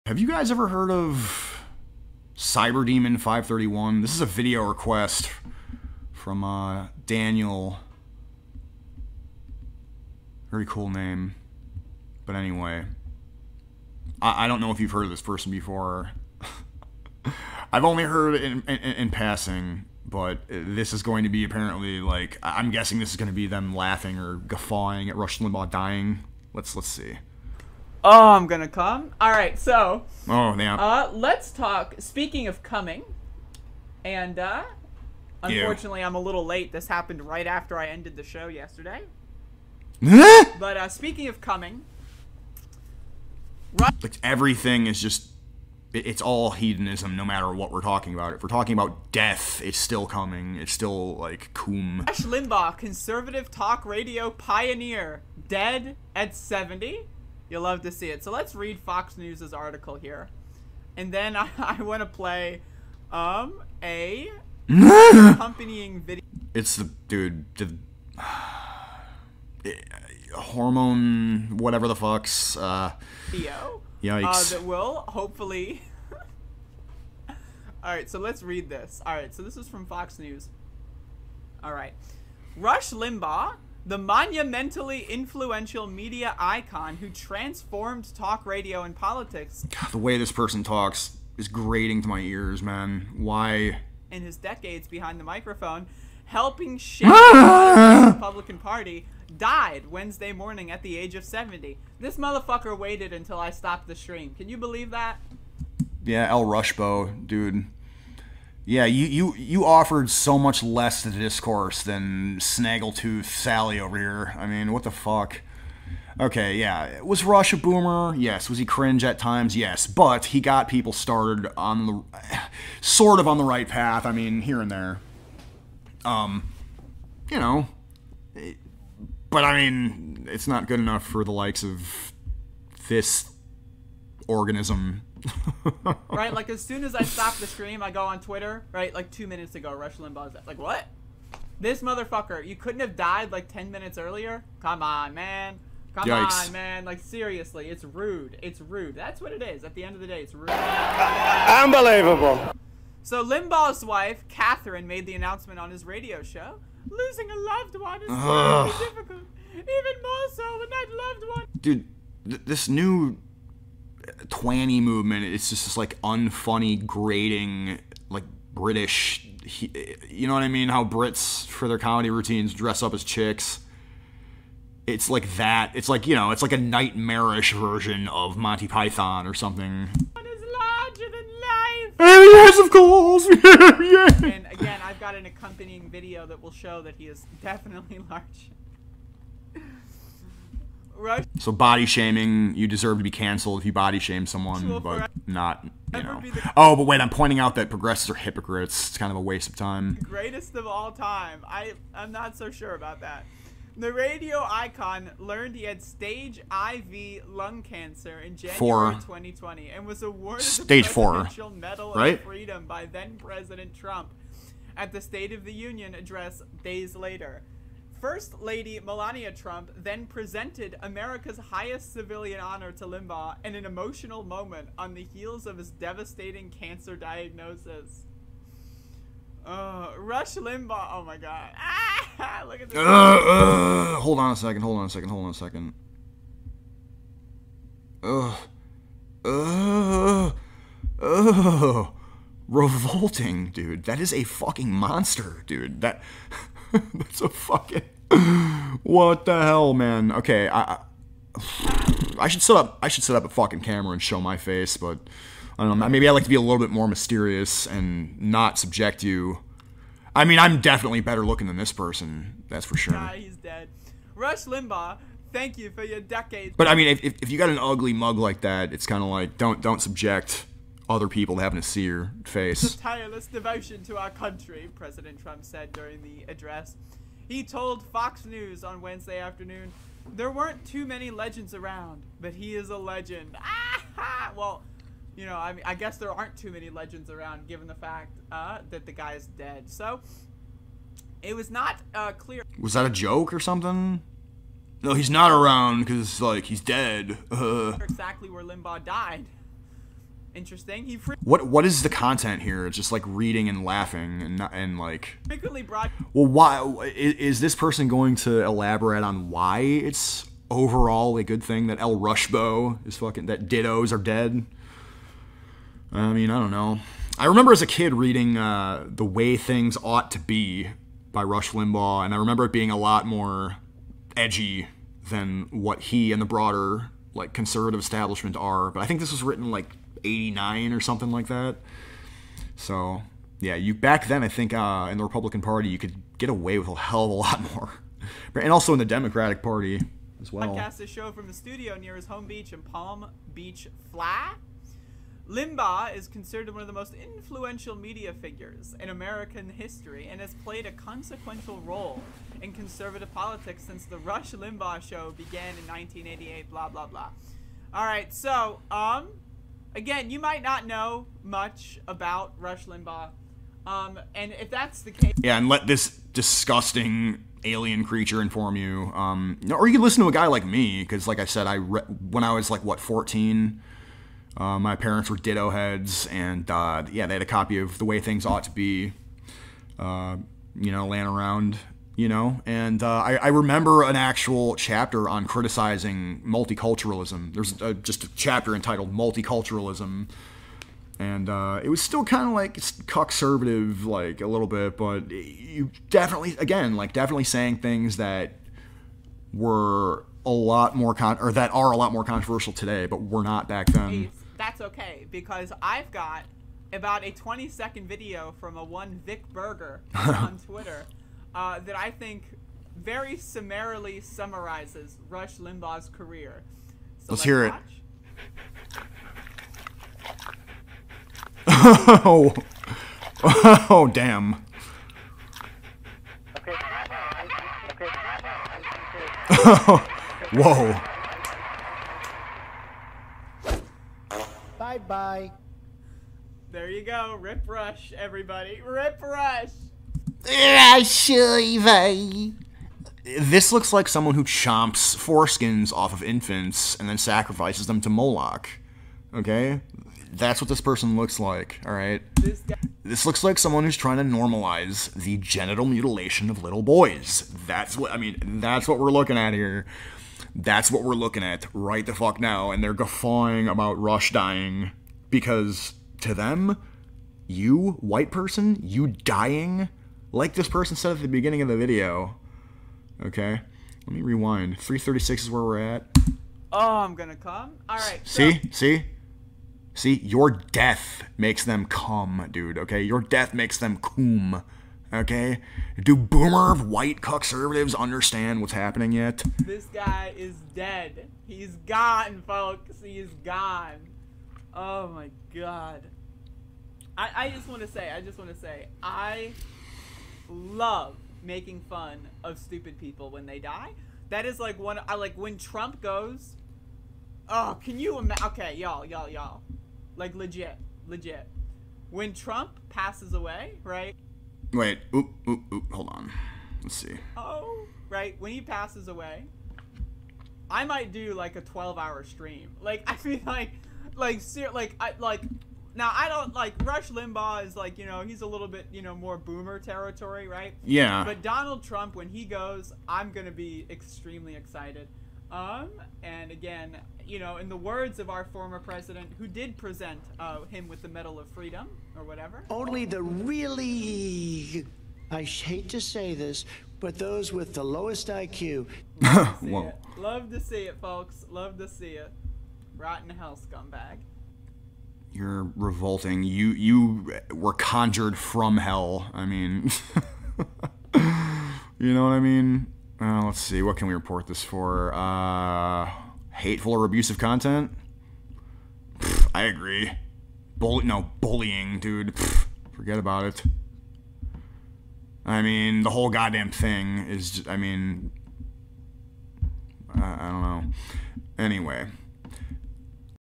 4Skin Severing Demon Person Gloats About Rush Limbaugh's Death
This is what reactions of disgust are made for.  Clipped from this stream.
BTC: 16DfhbdZD2EcDC8Gv82t66dm1zE1xDUboG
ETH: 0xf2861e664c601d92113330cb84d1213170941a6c
LTC: LZEZzxLdjzPUfUHJbXmvEhjduYQ5tn4FEs
4Skin Severing Demon Person Gloats About Rush Limbaugh's Death